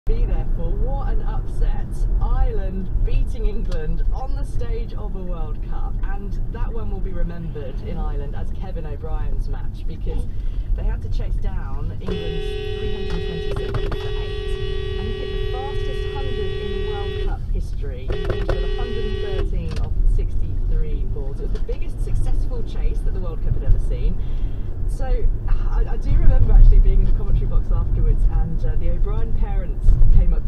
Bad Driving - KJ15LVO doesn't know how roundabouts work
The roundabout has clearly marked lanes - left lane for exits 1 & 2, right lane for exits 3 & 4. This Volvo KJ15LVO decides the rules don't apply to him, straightlines the roundabout, and doesn't bother indicating at any point.